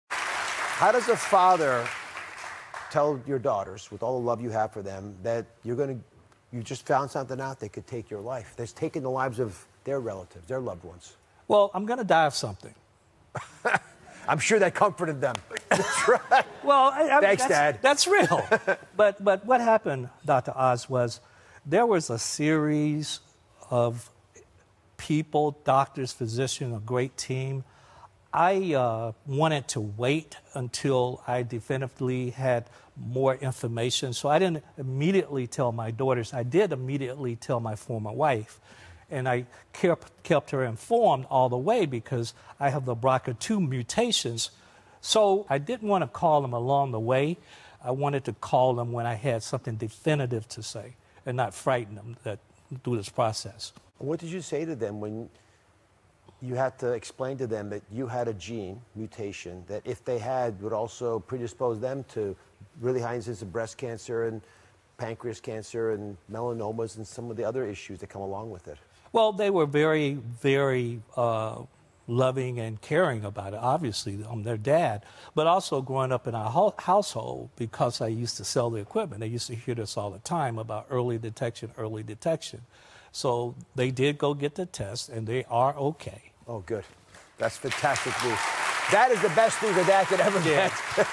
Watch: Matthew Knowles reveals how he told Beyoncé and Solange he had breast cancer
Matthew Knowles is revealing how he told daughters Beyoncé and Solange about his breast cancer diagnosis.

(ZoCo Productions, LLC)